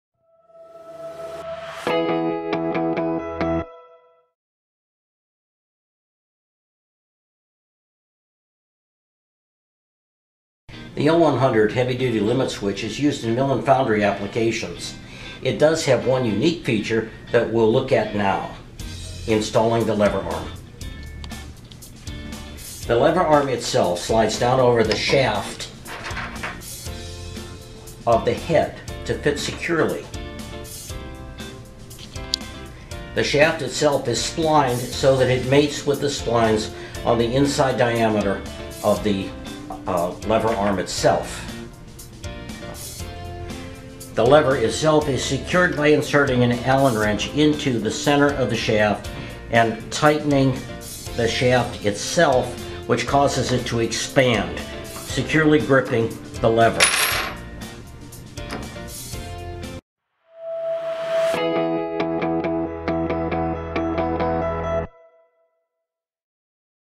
Installing the Lever Arm on a Square D Denison L100 Lox Switch | Schneider Electric Support
Tutorial for installing the lever arm to the switch body of a Square D® R.B. Denison® L100 Type L heavy duty Lox-Switch™ used in mill and foundry applications.